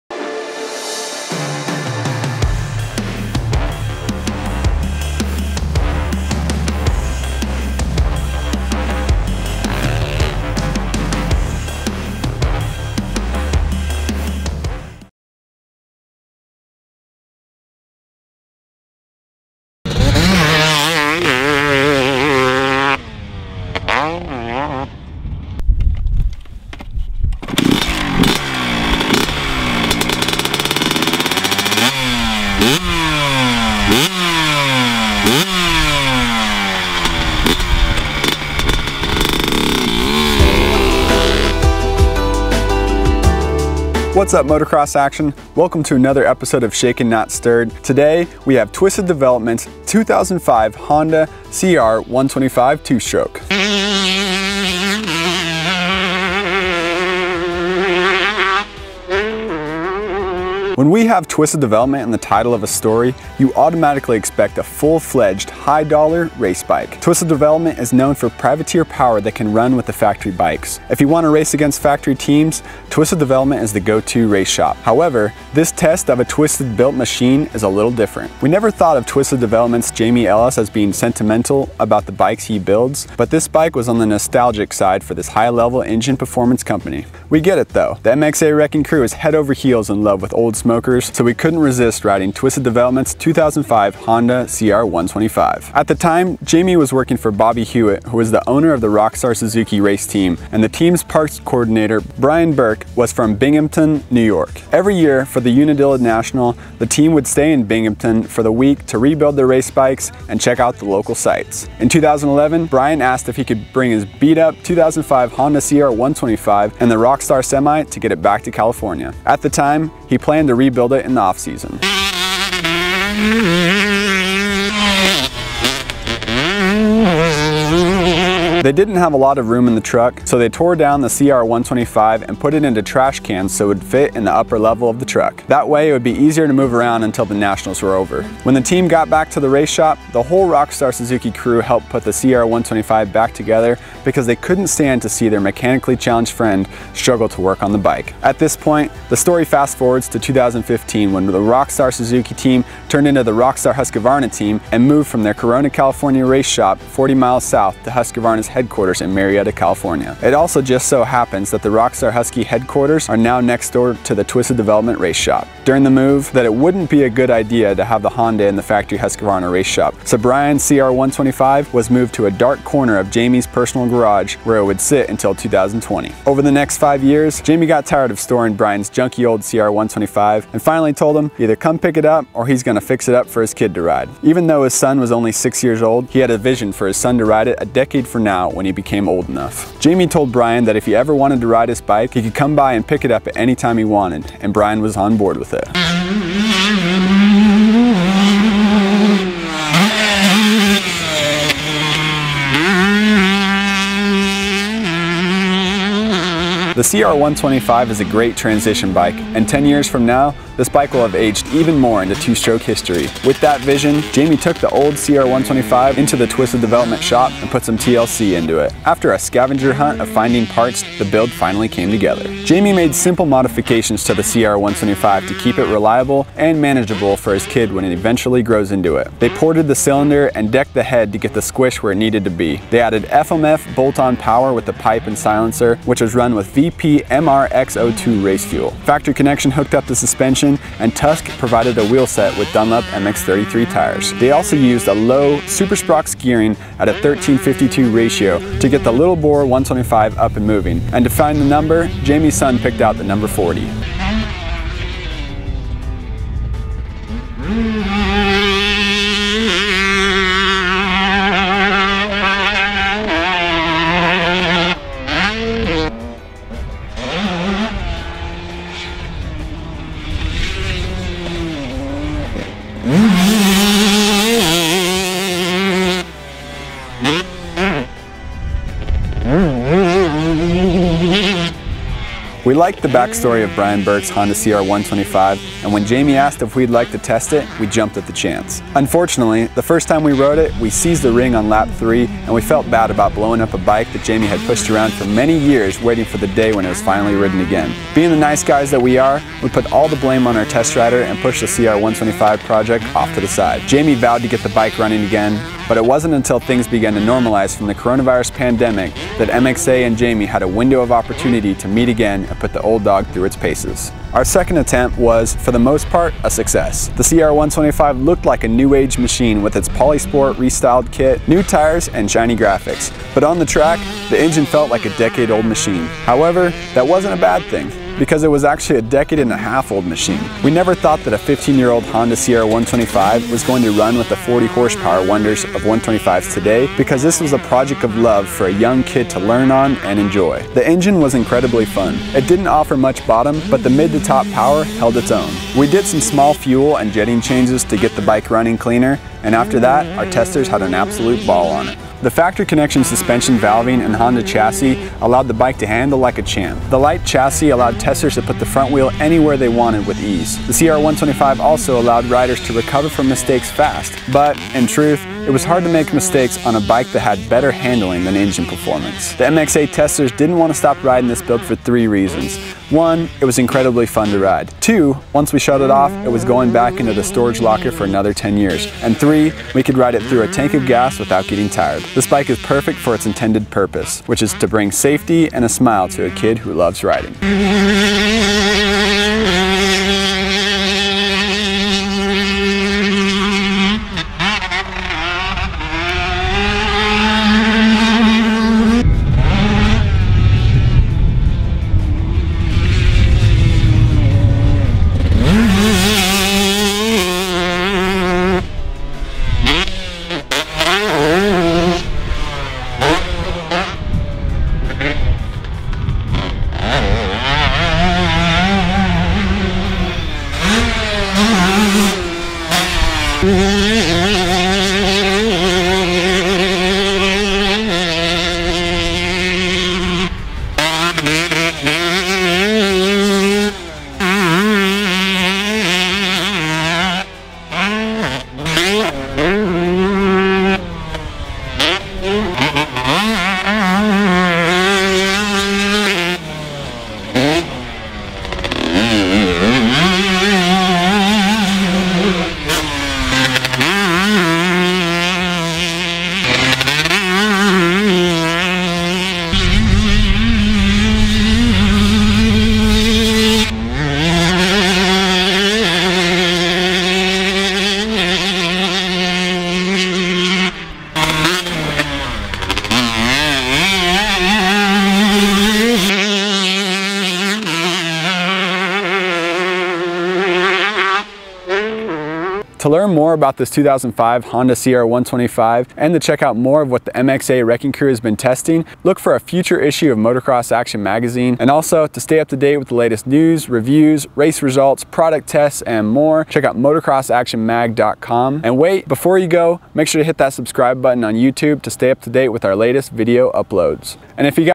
Twisted 2005 Honda CR125 Two Stroke Project - Motocross Action Magazine
When we have Twisted Development in the title of a story, you automatically expect a full-fledged, high-dollar race bike. Twisted Development (TD) is known for privateer power that can run with the factory bikes. If you want to race against the factory teams, Twisted Development is the go-to race shop; however, this test of a TD-built machine is a little different. We never thought of Twisted Development’s Jamie Ellis as being sentimental about the bikes he builds, but this bike was on the nostalgic side for this high-level engine performance company. We get it, though. The MXA wrecking crew is head over heels in love with old smokers, so we couldn’t resist riding Twisted Developments 2005 Honda CR125. The CR125 looked like a new-age machine with its Polisport restyled kit, new tires and shiny graphics, but on the track, the engine felt like a decade-old machine. That wasn’t a bad thing, because it was actually a decade-and-a-half-old machine. We never thought that a 15-year-old Honda CR125 was going to run with the 40-horsepower wonder 125s of today, because this was a project of love for a young kid to learn on and enjoy. The engine was incredibly fun. It didn’t offer much bottom, but the mid-to-top power held its own. We did some small fuel and jetting changes to get the bike running cleaner. After that, our testers had an absolute ball on it. The Factory Connection suspension valving and Honda chassis allowed the bike to handle like a champ. The light chassis allowed testers to put the front wheel anywhere they wanted with ease. The CR125 allowed riders to recover from mistakes fast, but in truth, it was hard to make mistakes on a bike with better handling than engine performance. The MXA gang didn’t want to stop riding this build for three reasons: (1) It was incredibly fun to ride. (2) Once we shut it off, it was going to go back into the storage locker for another 10 years. (3) We could ride through a tank of gas without getting tired. This bike is perfect for its intended purpose, which is to bring safety and a smile to a kid who loves riding.